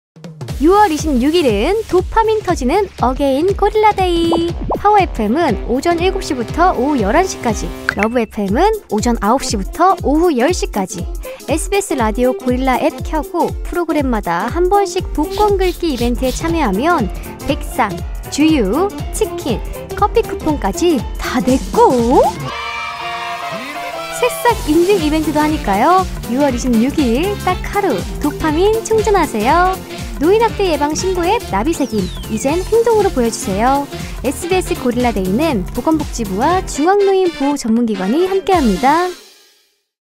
완전 럭키랄라잖아🍀 6월 26일 수요일! 선물이 쏟아지는 🐵고릴라 데이🐵🎁
🍀6월 26일 수요일! 🐵어게인 고릴라 데이🐵

3월에 왔던 고릴라 데이가 6월에 다시 찾아왔습니다🍀
SBS 라디오 공식 APP '고릴라'을 통해 이벤트 참여 가능합니다📢
많이 참여해서 총 1500만원 상당의 푸짐한 선물 받아 가세요➿🎁

‼️고릴라 데이 이벤트 참여 방법‼️
Step1. 휴대폰 or 컴퓨터에 고릴라 APP 설치📱💻
Step2. 고릴라 APP 접속 후 오른쪽 상단에 이벤트 배너 클릭!🖱️
Step3. 이벤트 페이지에서 행운 복권 긁기🪙
Step4. 당첨된 경품을 확인하고, 정보를 입력📝

✔️ 이벤트 운영 프로그램📻
💙파워FM : 김영철의 파워FM ~ 배성재의 텐
🩷러브FM : 이숙영의 러브FM ~ 그대의 밤, 정엽입니다
✔️기회는 프로그램별로 단 한 번 뿐! (러브FM/파워FM 동시 참여 가능✌️)
✔️더 자세한 사항은 아래 링크 참고 부탁드립니다!